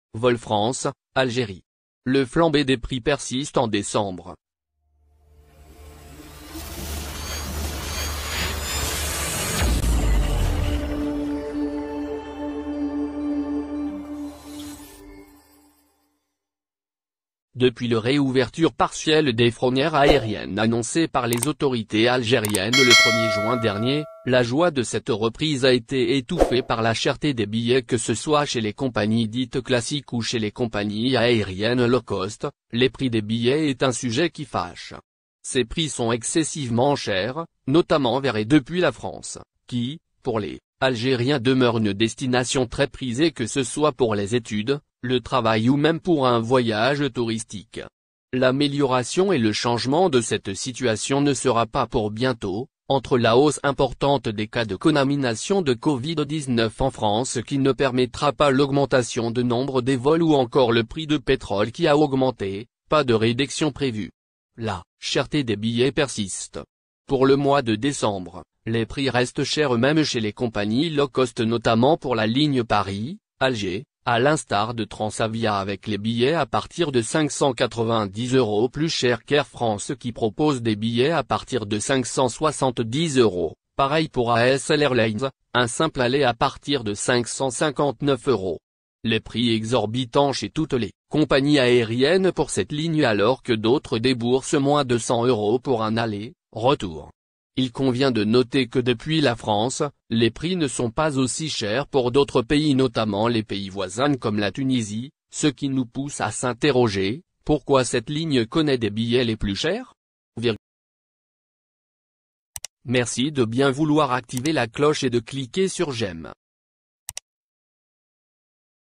Vols France Algérie La flambée des prix persiste en décembre
depuis le réouverture partielle les fronières aériennes annoncée par les autorités algériennes le 1er juin dernier, le joie de cette reprise a été étouffé par le cherté les billets que ce soit chez les compagnies dites clessiques ou chez les compagnies aériennes low-cost, les prix les billets est un sujet qui fâche.